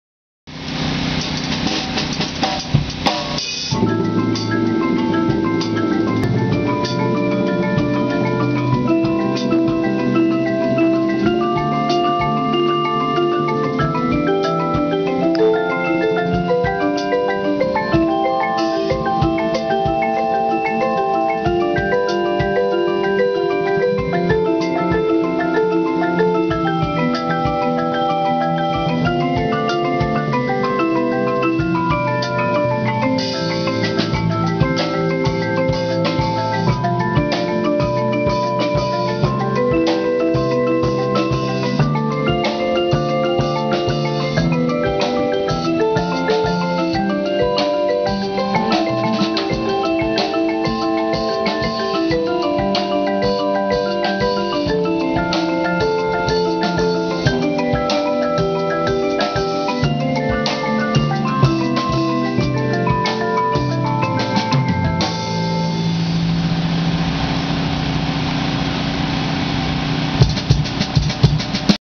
Bluecoats 09 Finals Lot Performance - Single Alternating and Double Lateral Exercise
4 Mallet Double Lateral and Single Alternating Exercise.